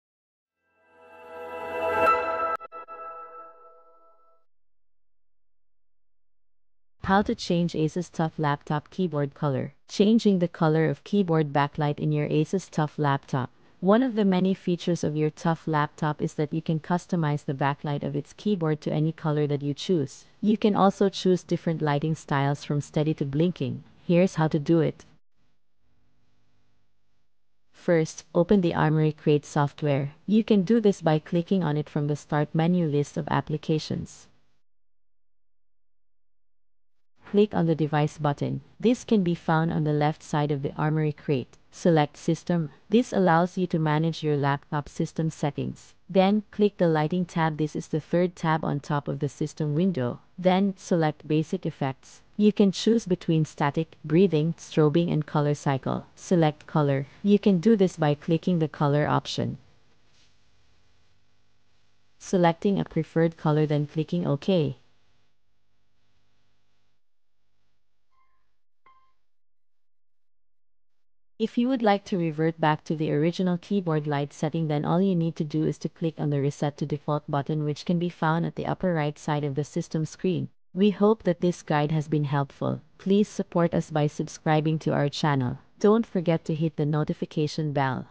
How To Change Asus TUF Laptop Keyboard Color
If you want change the keyboard color of your Asus TUF laptop, our video guide will show you ways on how to do it.